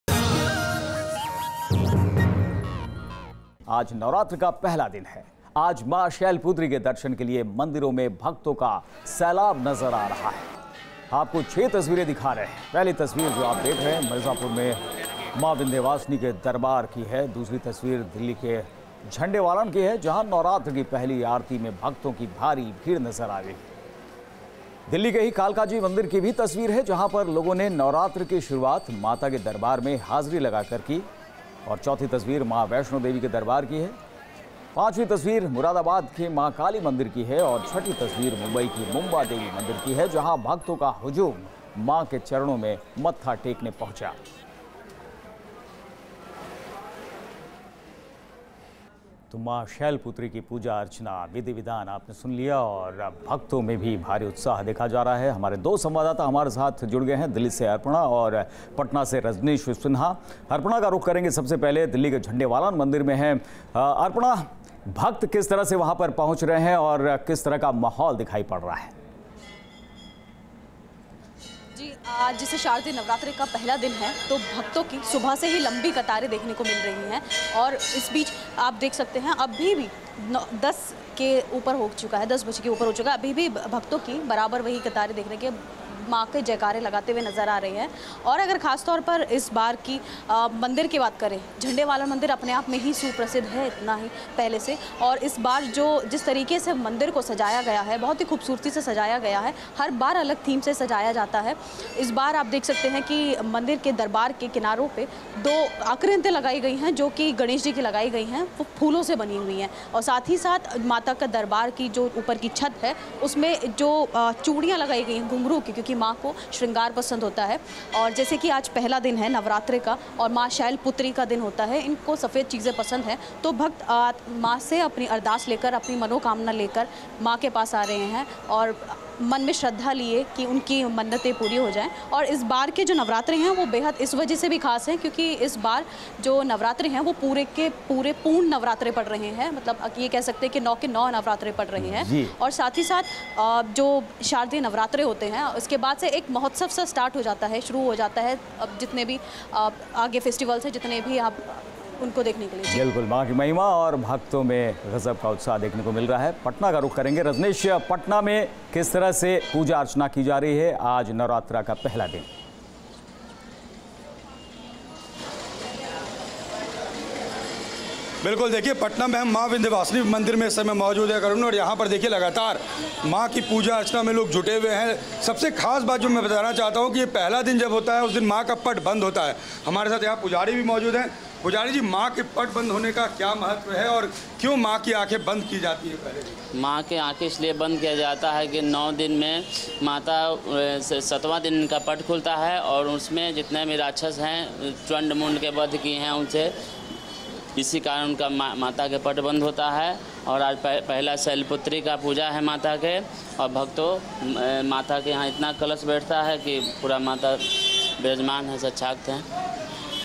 Navratri 2017: Worship Maa Shailputri, first form amongst Navdurga
On the first day of Navratri, Maa Shailputri is worshiped. The nine days represent the nine manifestations of Goddess Durga. Maa Shailputri has a half moon in her forehead. She is holding a trident in her right hand and lotus flower in her left hand. She rides on Nandi a bull.